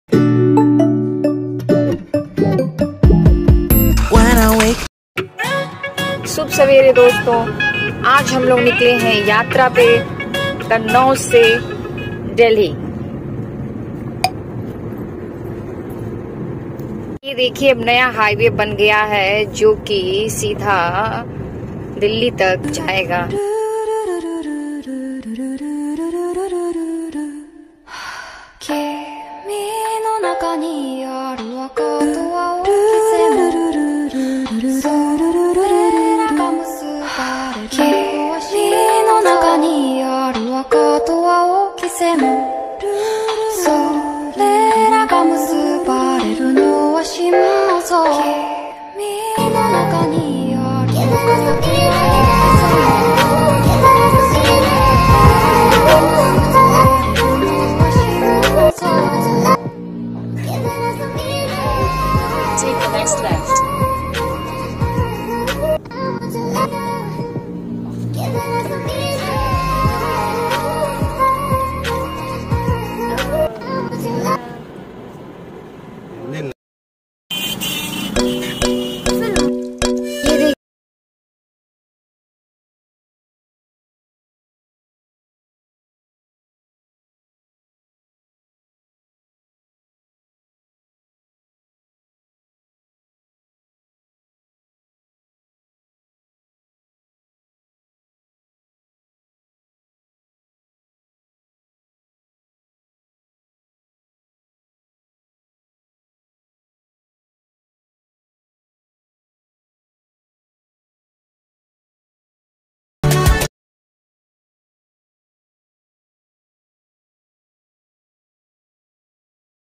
ll Kannauj TO Delhi ll by NH 34 ll Part 1 ll
kannauj to delhi with in 5 hours by car NH 34
tour of all historical place of Delhi